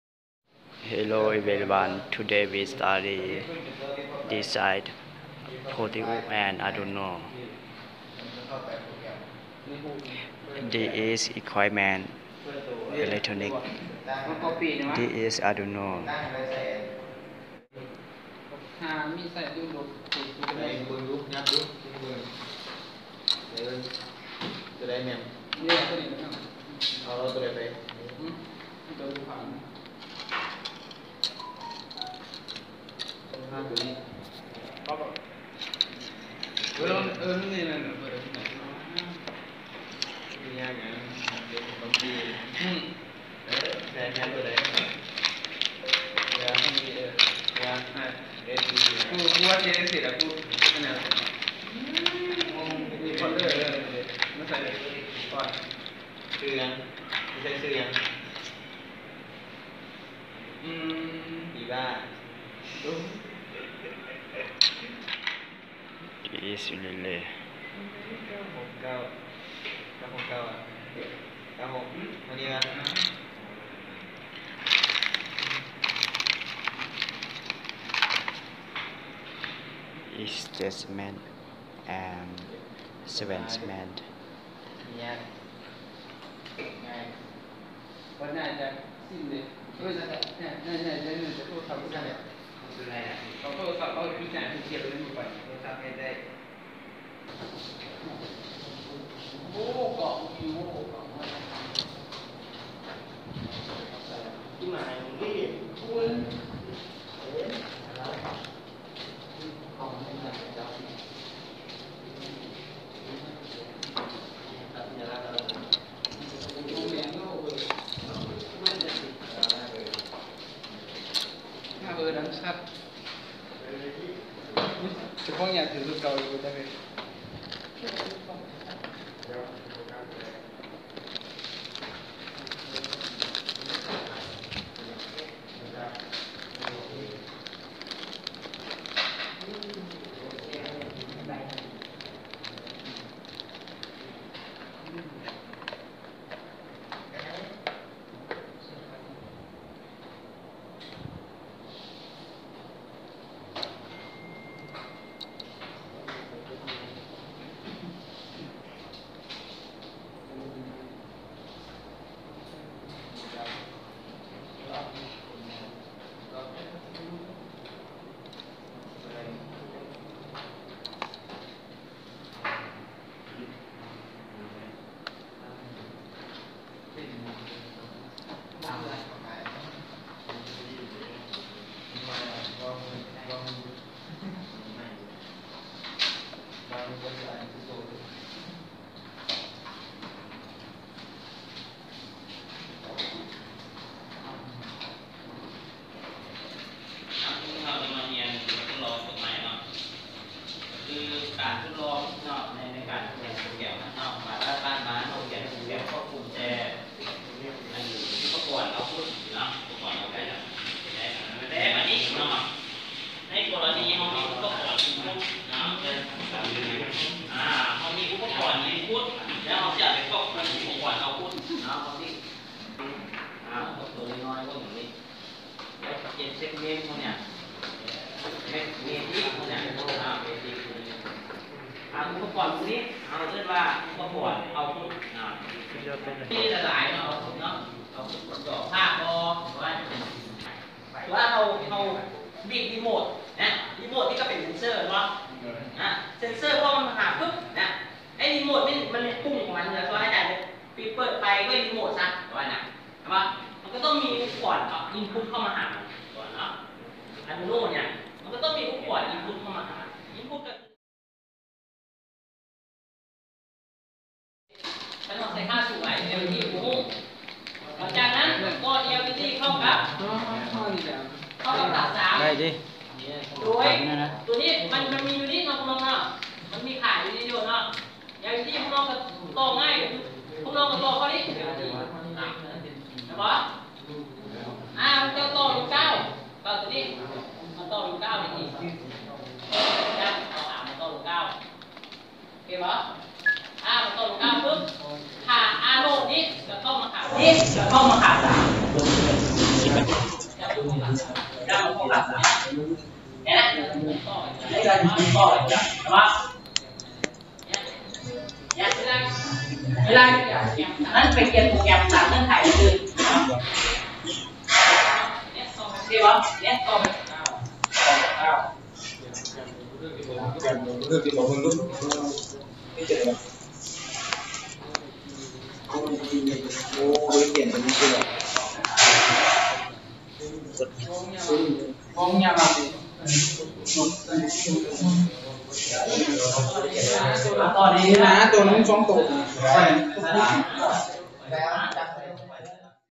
Basic design and experiment ELD in Arduino and Proteus
This is video basic design and experiment LED in Arduino and Proteus, Arduino control Led, using Arduino, baord, Led, Ressitor